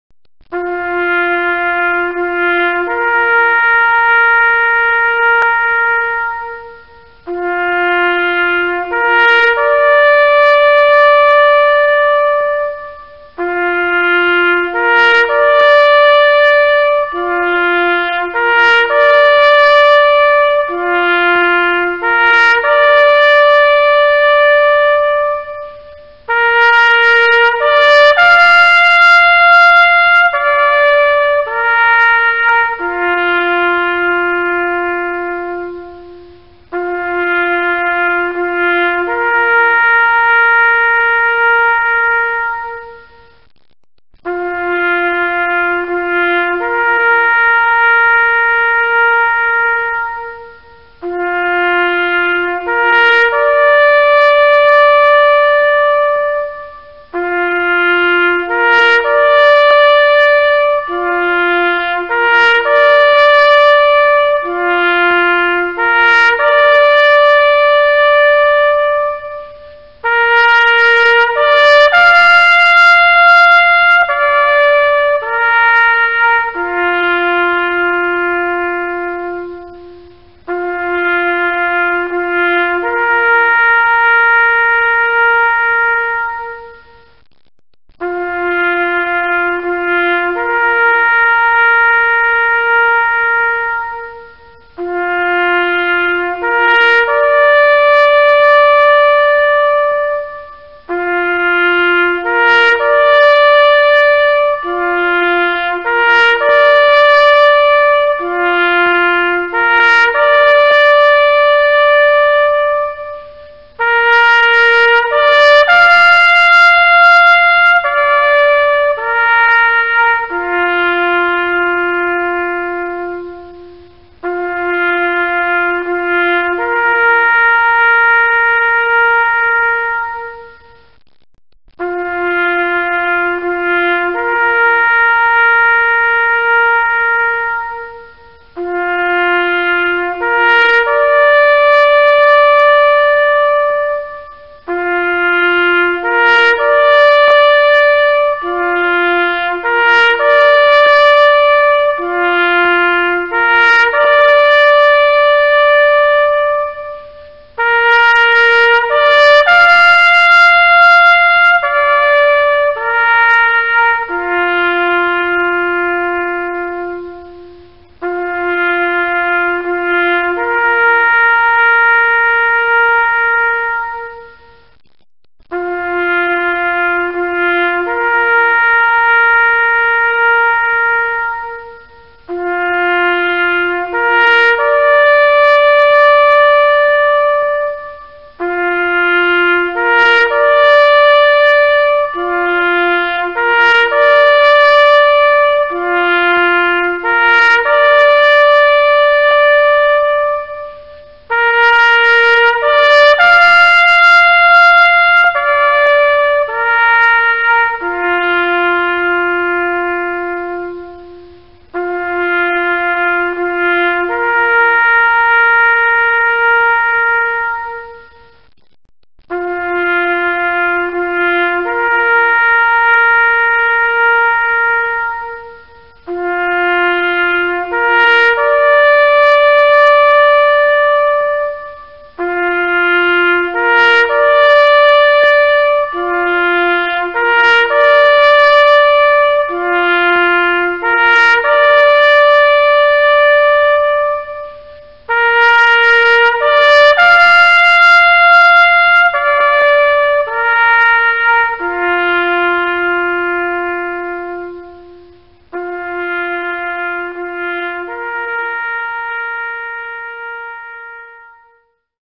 HISTORIA DEL SILENCIO MILITAR - EDITADO DE UN PowerPonint que recibí por email.
HISTORIA DEL SILENCIO MILITAR - ESTA HISTORIA LLEGÓ A MI A  TRAVÉS DE UN ARCHIVO PowerPoint, LA EDITÉ Y LA SUBO PARA COMPARTIR CON MIS SUSCRIPTORES